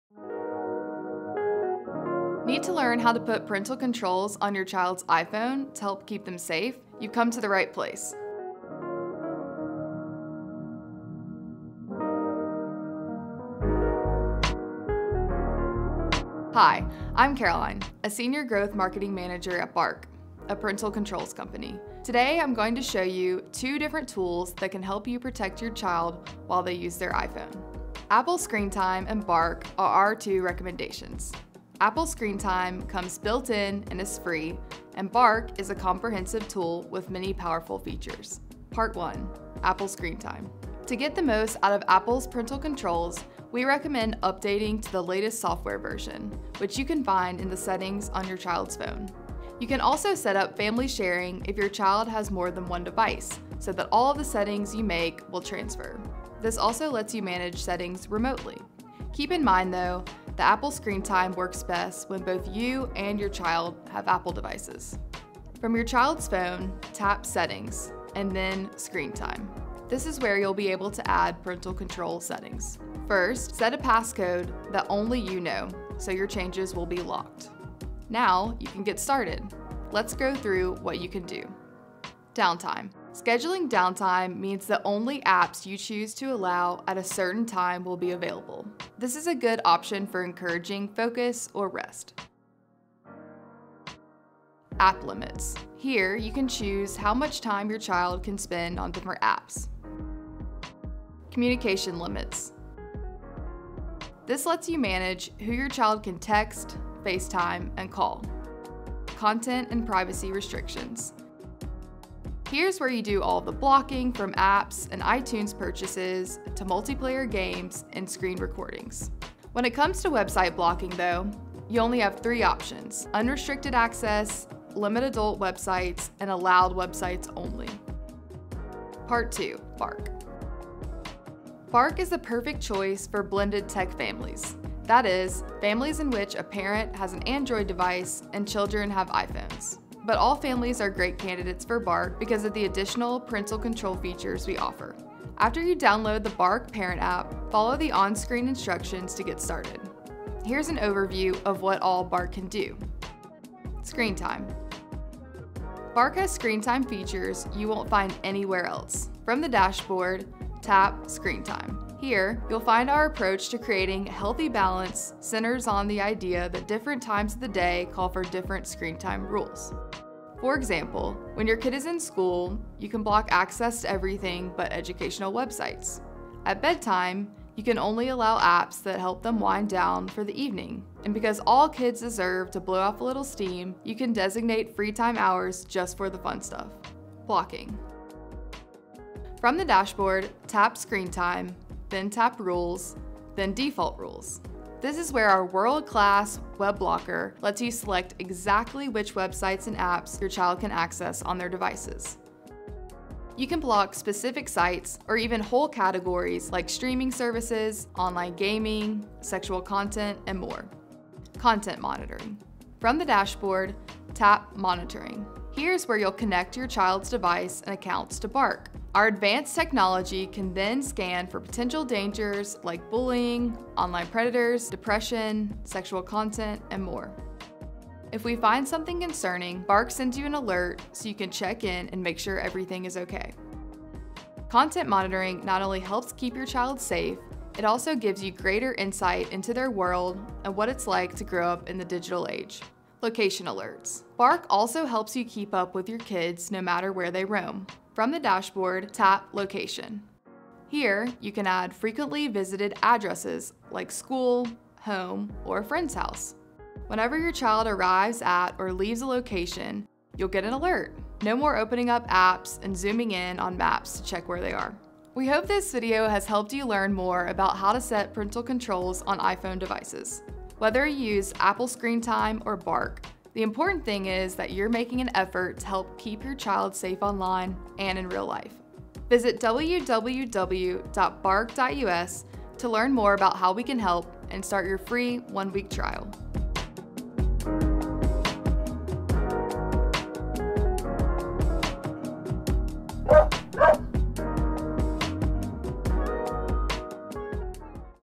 How to Put Parental Controls on iPhone Devices: An Overview for Families
Check out our video and learn how to put parental controls on iPhone devices to keep your children safe online.

In this video, You'll learn how to set parental controls on iPhone devices using the native iOS parental control app, Apple Screen Time, as well as Bark, a comprehensive online safety app.

Apple Screen Time features:
Downtime
Scheduling downtime means that only apps you choose to allow will be available. This is a good option for encouraging focus or rest.

App limits
Here, you can choose how much time your child can spend on specific apps or even whole categories of apps.

Communication limits
This lets you manage who your child can text, FaceTime, and call.

Content & privacy restrictions
Here’s where you do all the blocking, from apps and iTunes purchases to multiplayer games and screen recordings — there’s a lot to customize. When it comes to website blocking, though, you only have three options: unrestricted access, limit adult websites, and allowed websites only.

Bark features:

Screen time
Bark has screen time features you won't find anywhere else.  Our approach to screen time centers on the idea that different times of day call for different screen time rules.

Blocking
Our world-class web filter lets you select exactly which websites and apps your child can access on their devices. You can block specific sites — or even whole categories like streaming services, online gaming, sexual content, and more.

Content monitoring
Bark scans texts and social media for dangers like bullying, online predators, depression, sexual content, and more. If something concerning is found, Bark sends you an alert so you can check in and make sure everything is okay.

Location alerts
Bark also helps you keep up with your kids no matter where they roam.

to learn more about our service and start your free, one-week trial.